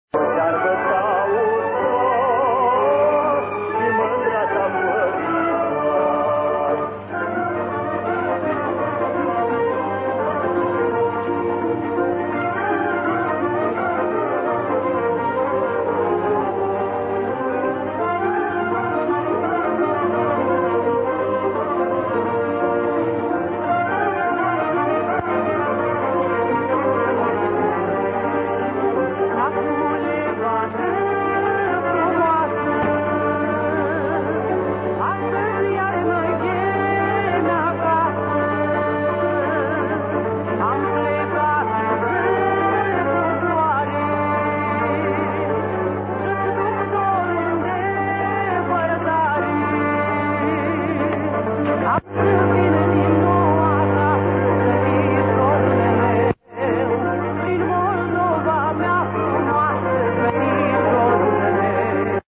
ETERE 14 - ZZZZZZZ - RADIO BUCAREST - SFRENATA DANZA E FINE DI ETERE 14 - AM RADIO SEPT-OCT 1992.flv
ETERE 14
  SETTEMBRE-OTTOBRE 1992
  NASTRO: SONY 60 MINUTI

RICEVITORE: KENWOOD R-5000
ANTENNA: SIRIO 2008 5/8 27 MHZ.